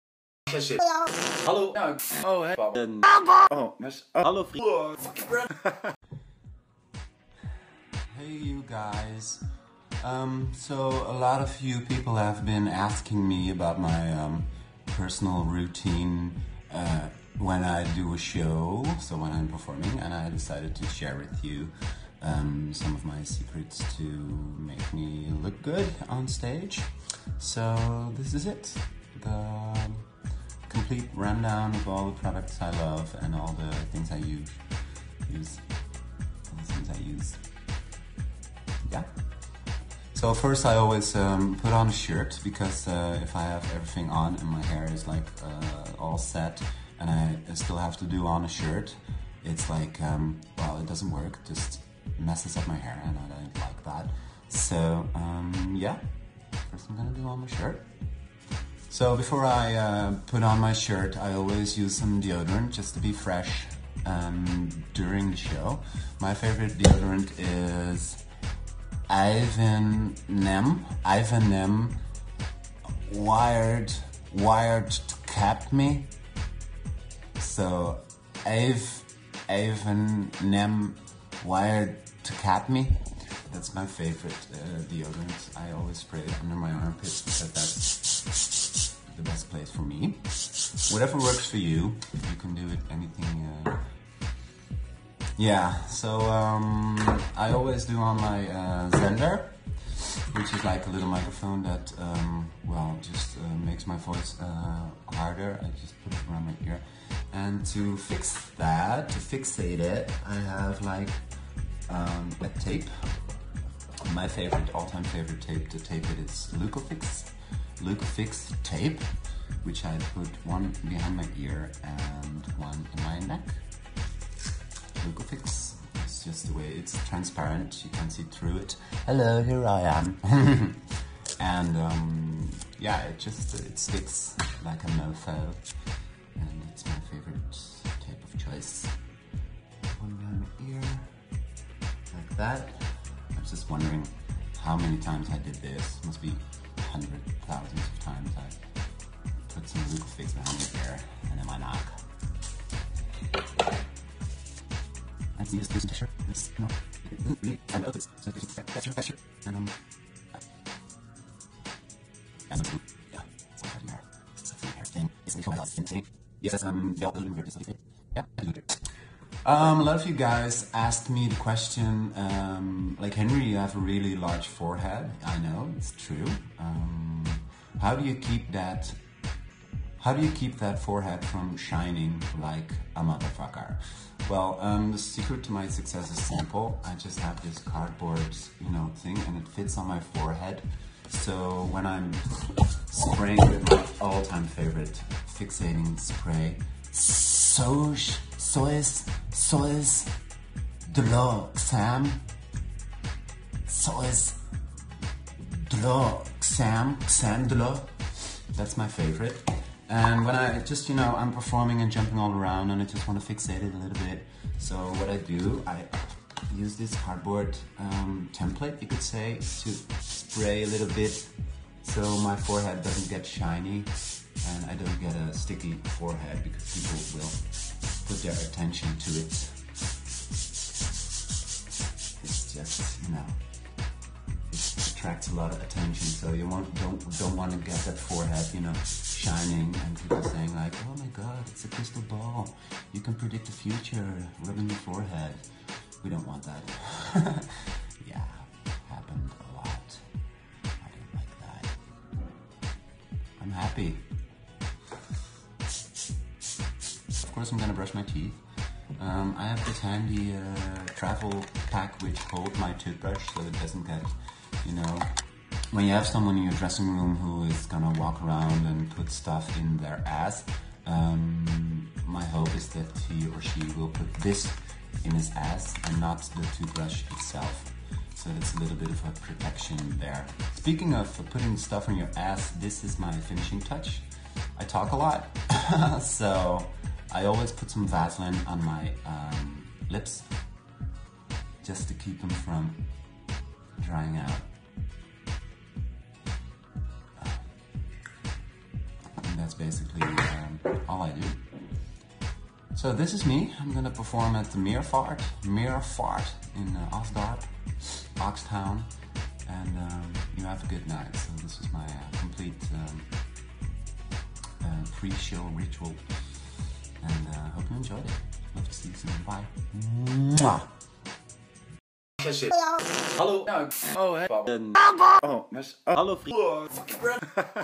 BEAUTY SECRETS | HENRY VAN LOON | VLOG #108
Complete pre-show preparation rundown

In this video i give you my complete pre-show preparation rundown. A lot of people asked me: “Henry, how do you prepare for a show, why do you look so good on stage? Which products do you use?” In this video i tell you all my secrets. ❤️